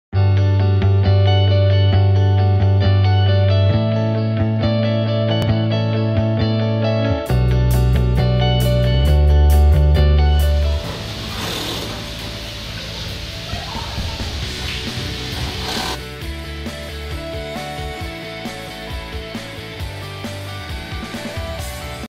Led Lamp Post sign high brightness waterproof group control available
Led Lamp Post sign, hot sale and hot production!

Features:
1. Wireless group control;
2. Waterproof cabinet;
3. High Definition;
4. Easy and reliable installation

TBC LED
Topbright Creation Limited
ADD: Room403, Building B, Sanlian Industrial Zone, Shiyan Street ,Shenzhen, China